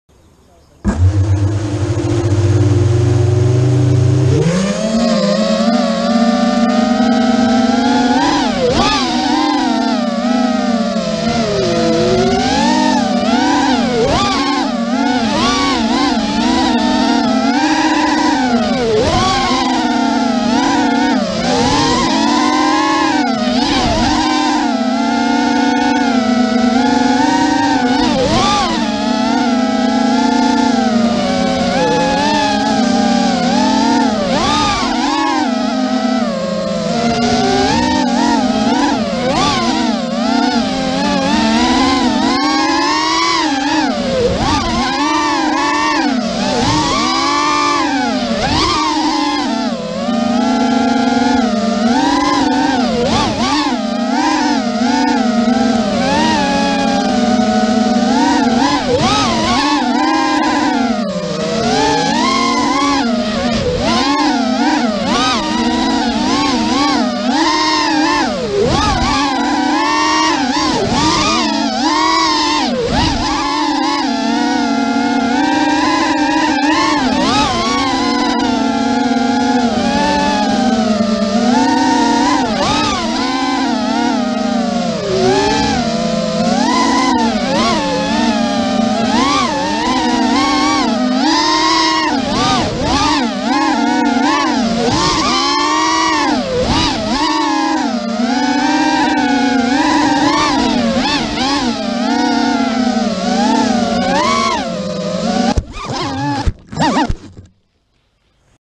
Racing at the Perotin Race Park_FPV dans le Grand Est
Too long run...

Specs :
QAV210
EMAX 2600kV
LittleBee 30A, BlHeli 14.5
Naze32 rev6
Betaflight 2.7
SBUS looptime 250
DAL 5045x3BN V2 props